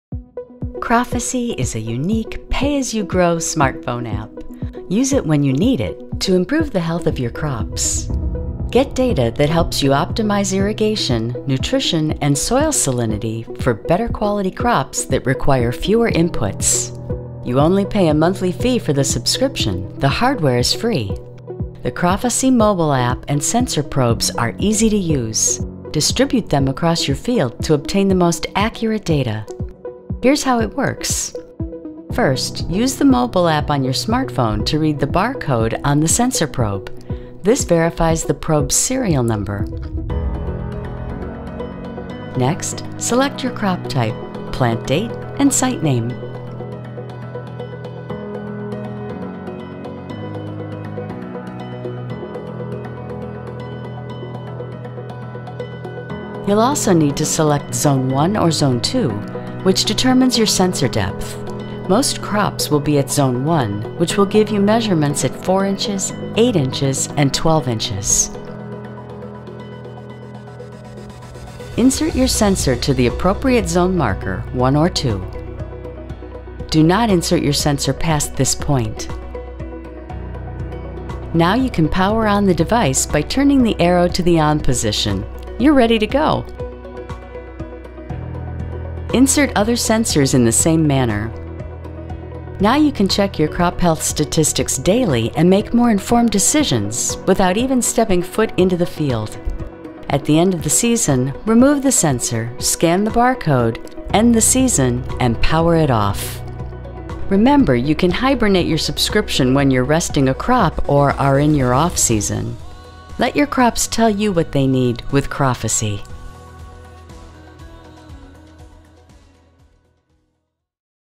Introducing Crophesy Pay as you Grow Crop Monitoring by AquaSpy
Use Crophesy when you need it to save water and optimize crop yield.